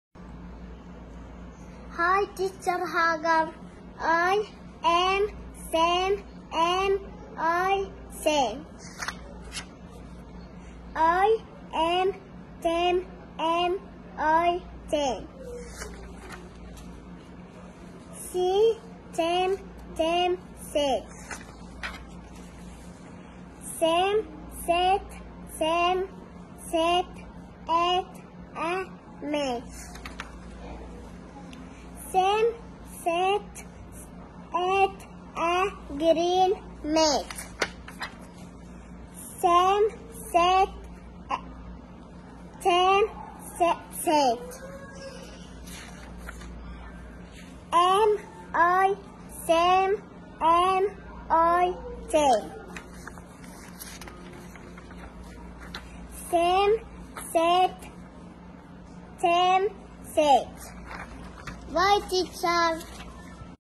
Reema_International_School Reading 📕 🌺KG3A 🌺
🌷Sleep is good...and books are better."
🌷 "The more that you read 📕the more things you will know.
🌷The more that you learn, the more places you'll go,"
Well done Leena 👏👏👏
🌺KG3A 🌺
👩‍💻T.Hagar 👩‍💻